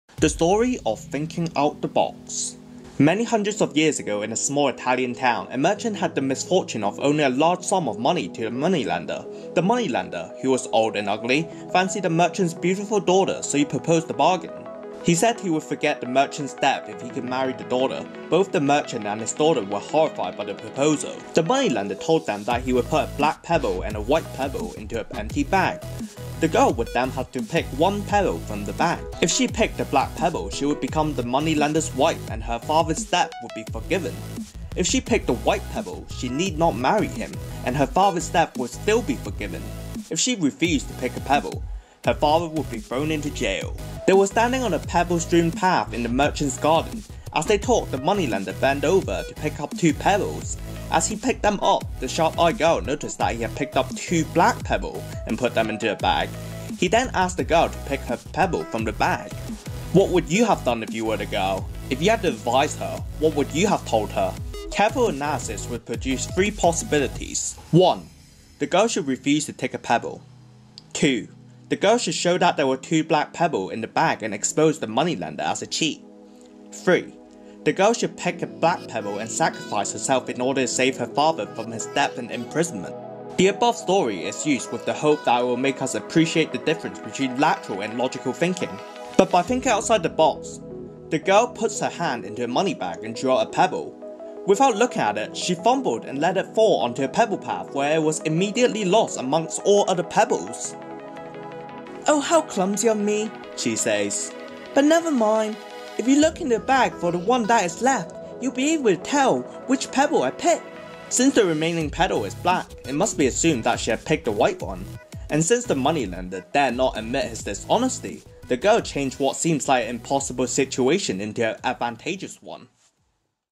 THINKING OUT THE BOX - SHORT ANIMATED STORY
Thinking "Out of the Box"

Many hundreds of years ago in a small Italian town, a merchant had the misfortune of owing a large sum of money to the moneylender. The moneylender, who was old and ugly, fancied the merchant's beautiful daughter so he proposed a bargain. He said he would forget the merchant's debt if he could marry the daughter. Both the merchant and his daughter were horrified by the proposal.

The moneylender told them that he would put a black pebble and a white pebble into an empty bag. The girl would then have to pick one pebble from the bag. If she picked the black pebble, she would become the moneylender's wife and her father's debt would be forgiven. If she picked the white pebble she need not marry him and her father's debt would still be forgiven. But if she refused to pick a pebble, her father would be thrown into jail.

They were standing on a pebble strewn path in the merchant's garden. As they talked, the moneylender bent over to pick up two pebbles. As he picked them up, the sharp-eyed girl noticed that he had picked up two black pebbles and put them into the bag. He then asked the girl to pick her pebble from the bag.

What would you have done if you were the girl? If you had to advise her, what would you have told her? Careful analysis would produce three possibilities:

1. The girl should refuse to take a pebble.
2. The girl should show that there were two black pebbles in the bag and expose the moneylender as a cheat.
3. The girl should pick a black pebble and sacrifice herself in order to save her father from his debt and imprisonment.

The above story is used with the hope that it will make us appreciate the difference between lateral and logical thinking.
But by thinking out the box the girl put her hand into the moneybag and drew out a pebble. Without looking at it, she fumbled and let it fall onto the pebble path where it immediately became lost among all the other pebbles.

"Oh, how clumsy of me," she said. "But never mind, if you look into the bag for the one that is left, you will be able to tell which pebble I picked."

Since the remaining pebble is black, it must be assumed that she had picked the white one. And since the moneylender dared not admit his dishonesty, the girl changed what seemed an impossible situation into an advantageous one.

MORAL OF THE STORY: Most complex problems do have a solution, sometimes we have to think about them in a different way.